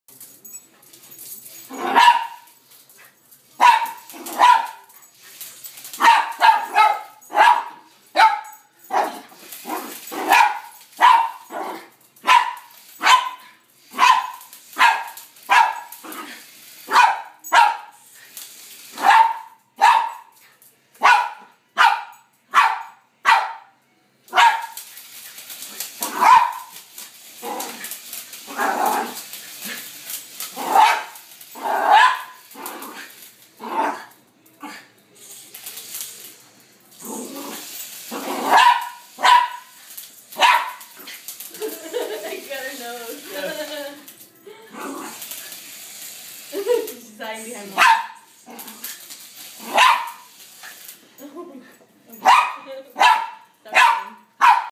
KATY PERRY POODLE TAKES DOWN A HELICOPTER
Poodle dressed like Katy Perry Trys to take down a Helicopter